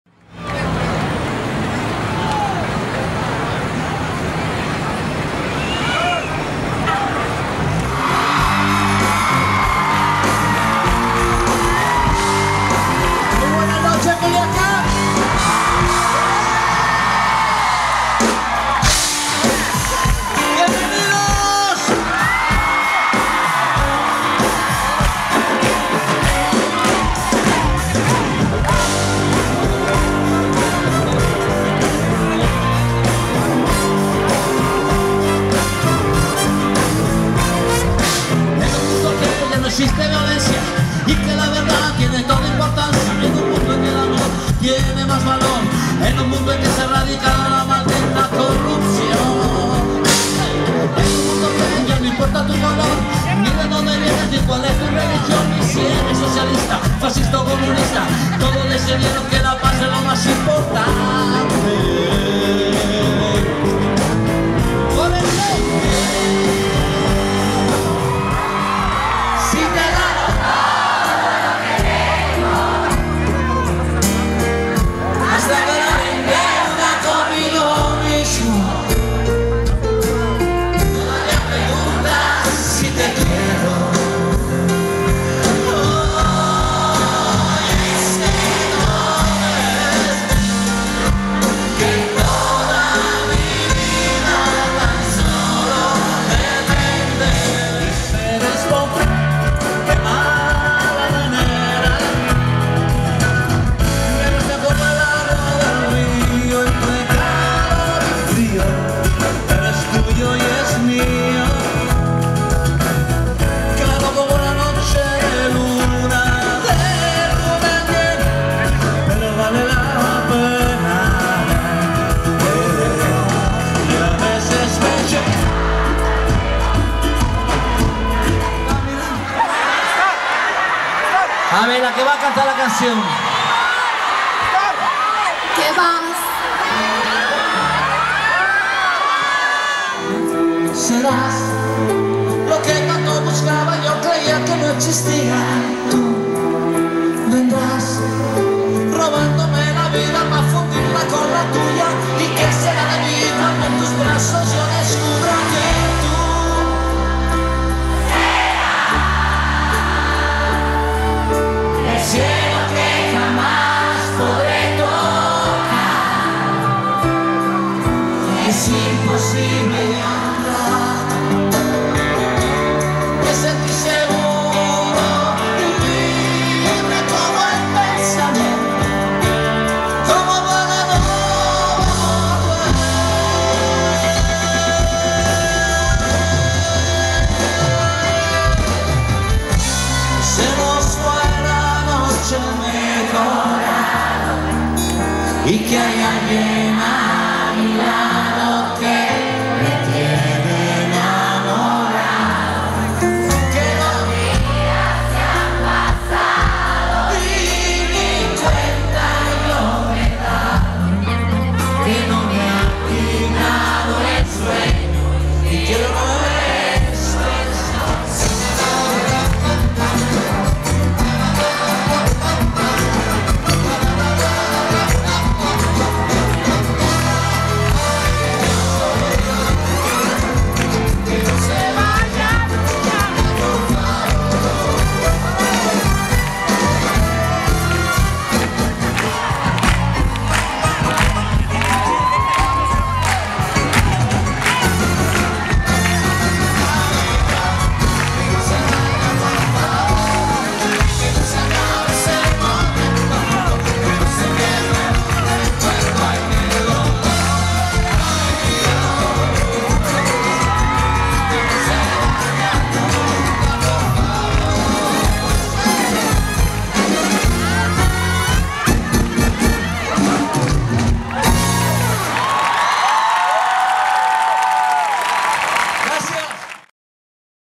Franco de Vita @Festival Internacional Universitario de la Cultura
Vibrante y emocionante concierto de
Franco de Vita en Culiacán.